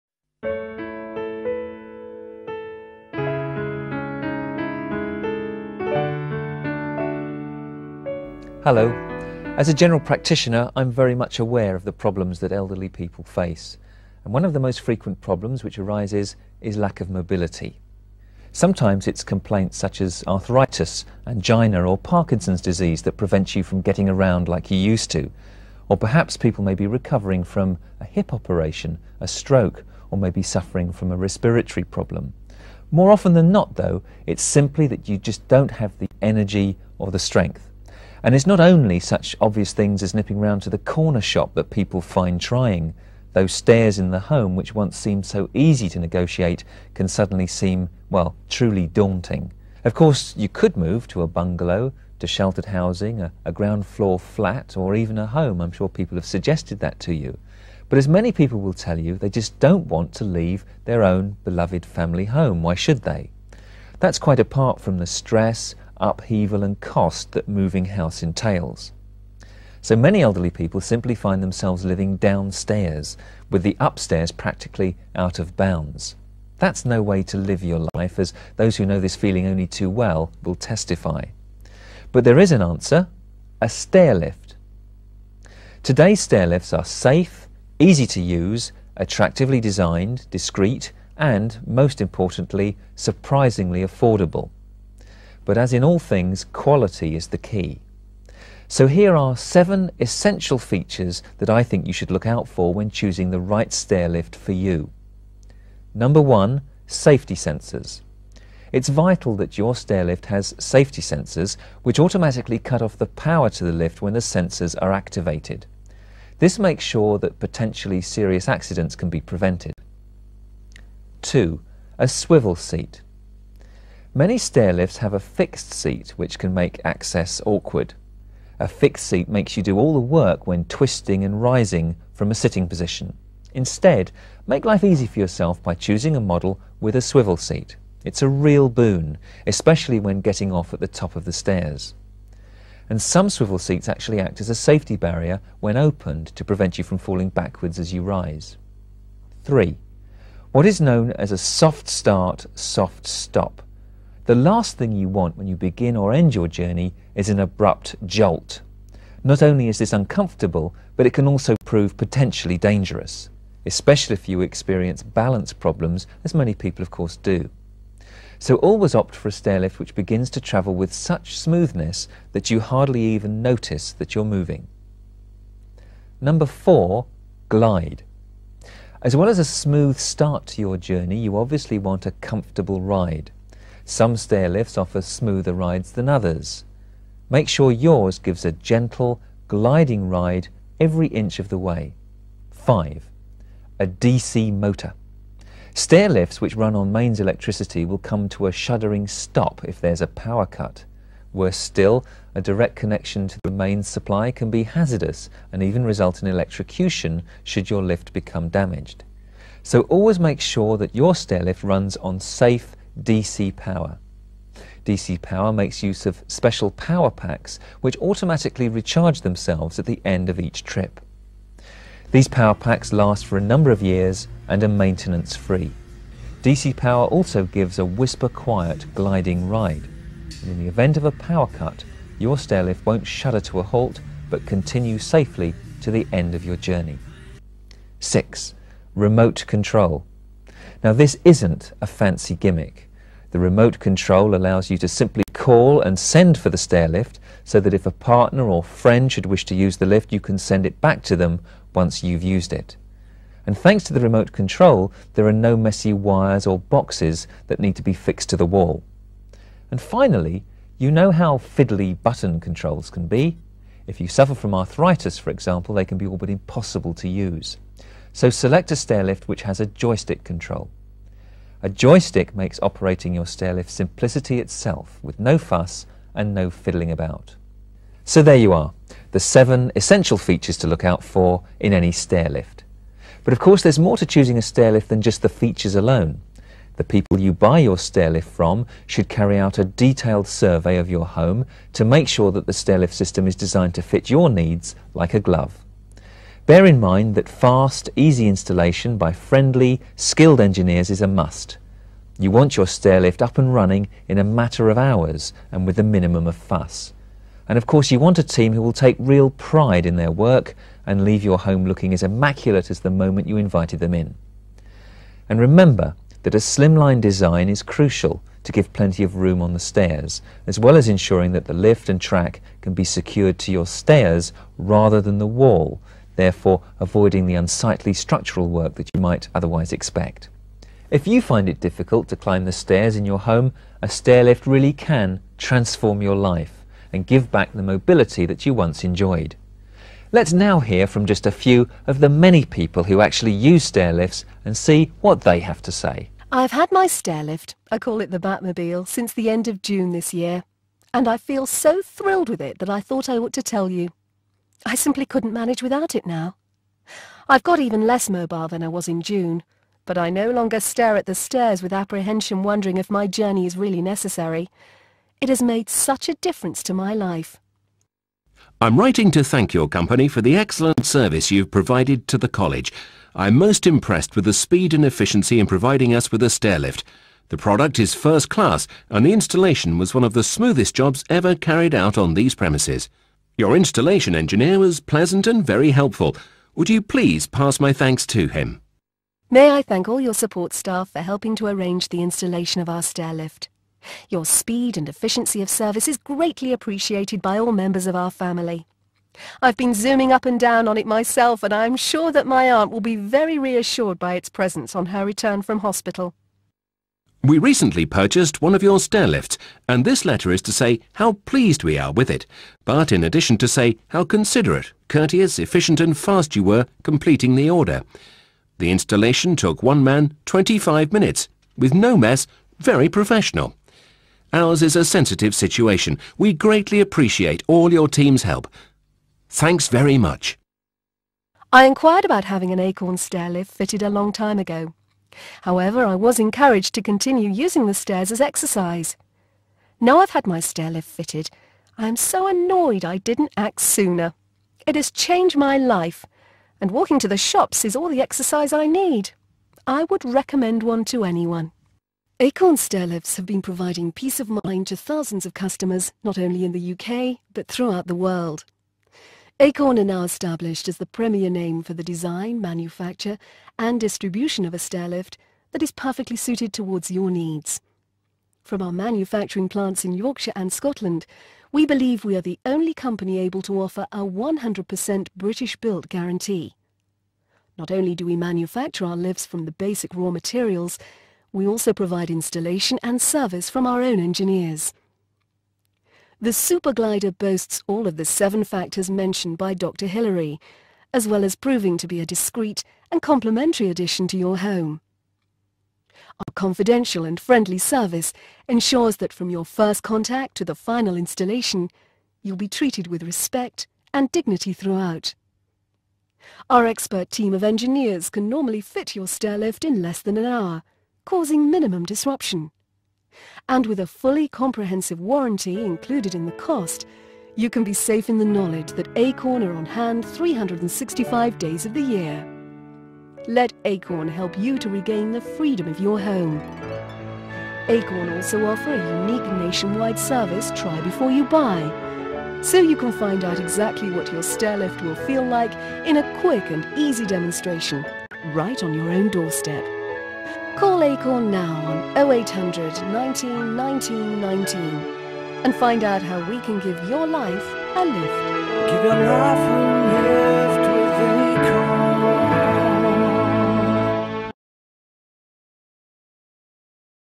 Acorn Stairlifts Information Video With Dr Hilary Jones
Acorn Stairlifts - Dr. Hilary Jones talks about what to look for when buying a Stairlift. for more information.